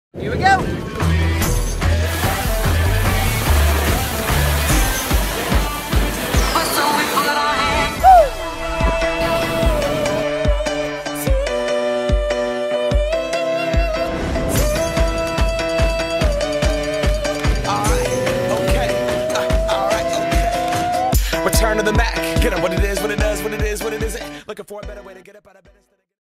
DOUBLE BLACK DIAMOND SKIING with Ryan Hellquist!
Ryan Hellquist can ski down double black diamonds and do jumps and hold a camera while skiing and can stop on a mark and say lines!